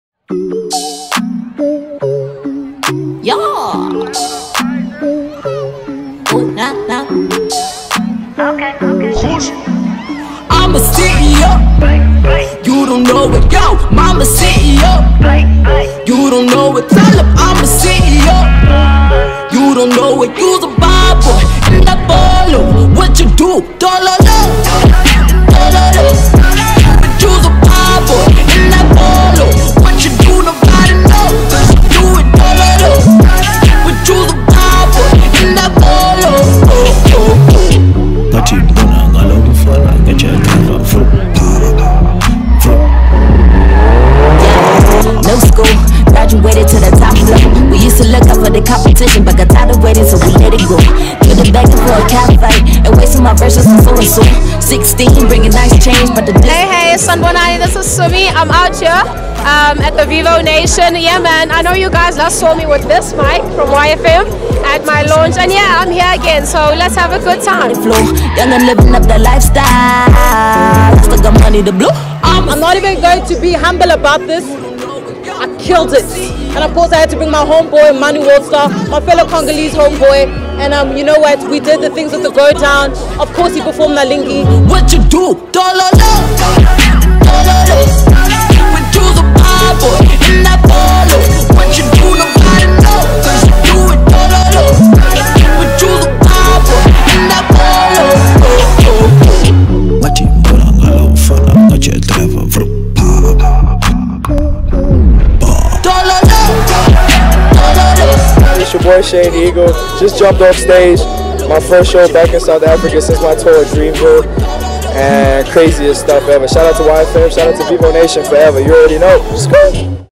"One of the greatest parties of the year!" | Vivonation 2019 Weekend
Here's a snippet of annual weekend event, Vivonation by Volkswagen, which has easily become one of the greatest parties of the year!

With performers like AKA, Simmy, Sho Madjozi, Mlindo The Vocalist, Rouge and Sjava - a total blast was to be expected.